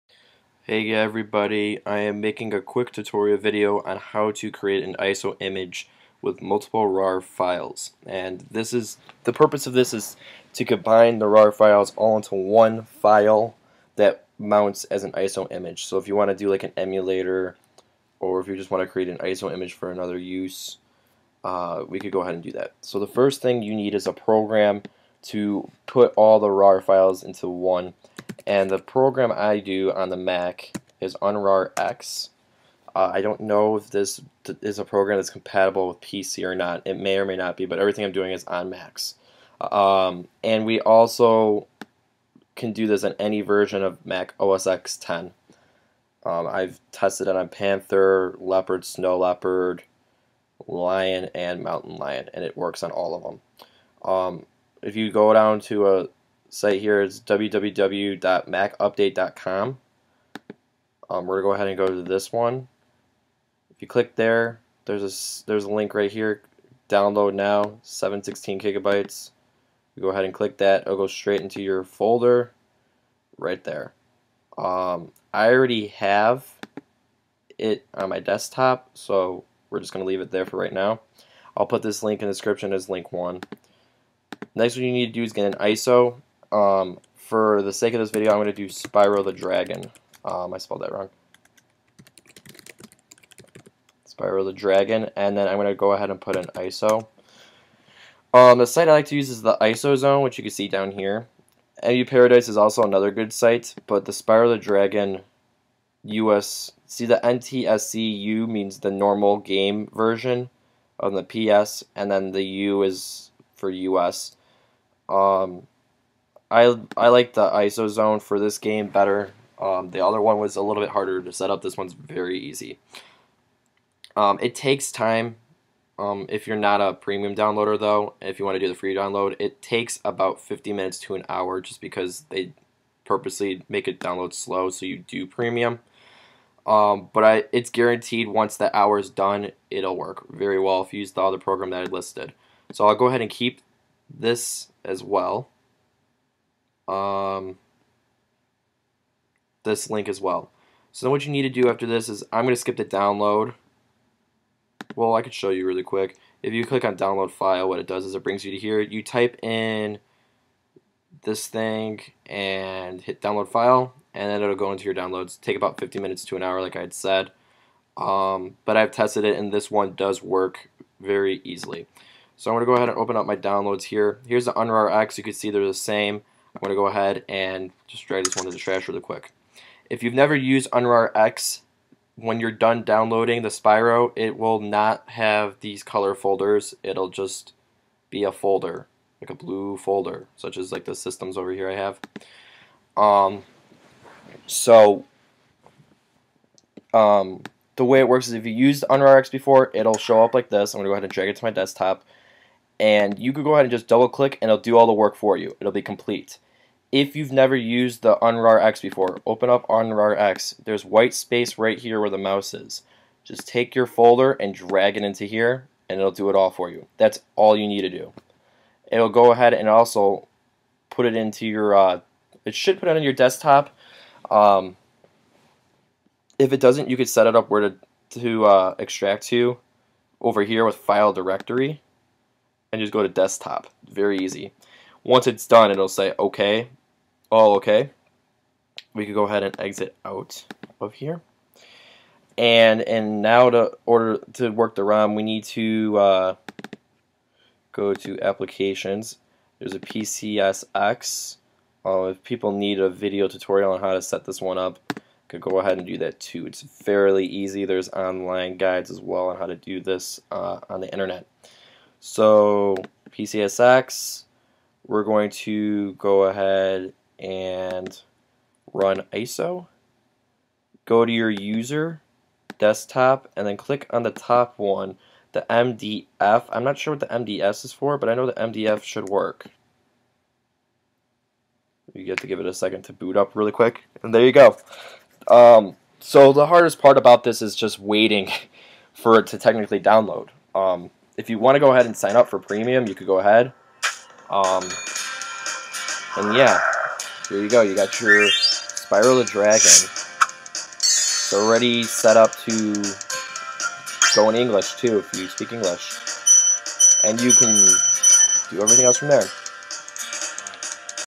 Create ISO Image with Multiple RAR Files on Mac OSX
Here I show how to take multiple RAR files and convert them into a SINGLE ISO image. This is a VERY useful guide for those who want to emulate video games and such.